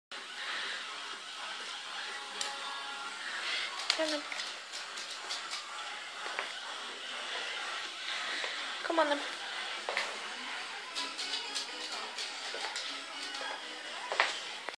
luxating patella, day after surgery
a short video of diesel one data after surgery